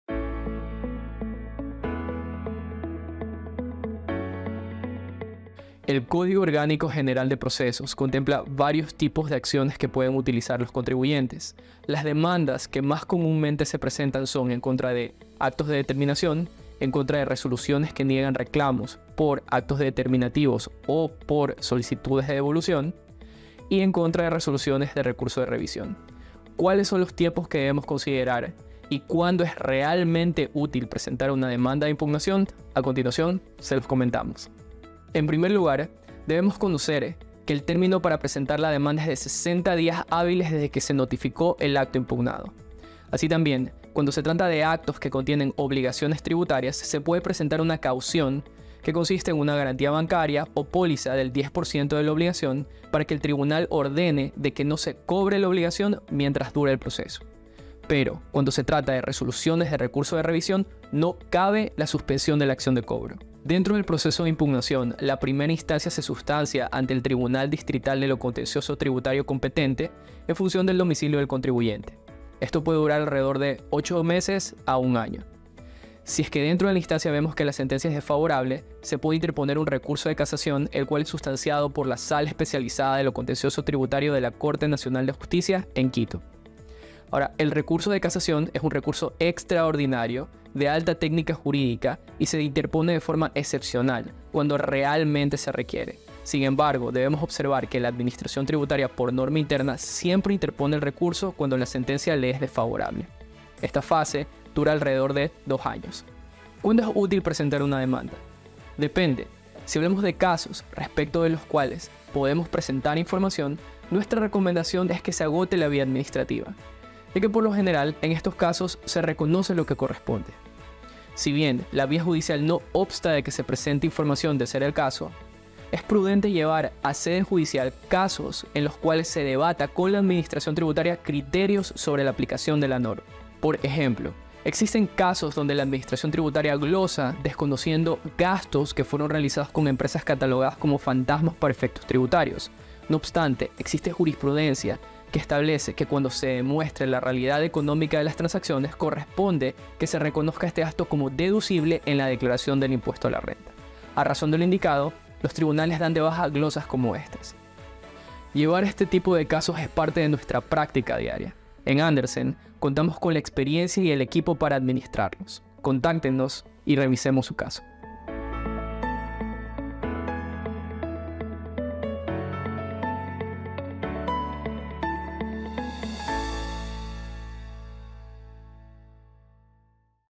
Aspectos generales sobre Litigios tributarios
📑 El Código Orgánico General de Procesos - COGEP, contempla varios tipos de acciones que pueden ser utilizadas por los contribuyentes. Las demandas, que más comúnmente se presentan, son en contra de:

✅ actos determinativos,
✅ resoluciones que nieguen reclamos por actos de determinación o por devolución de valores, y
✅ resoluciones de recurso de revisión

⏳ ¿Cuáles son los tiempos que se deben considerar y cuando es realmente útil presentar una demanda de impugnación? 🤔

En el siguiente video, nuestro Legal Manager Alfonso Ríos nos los comenta.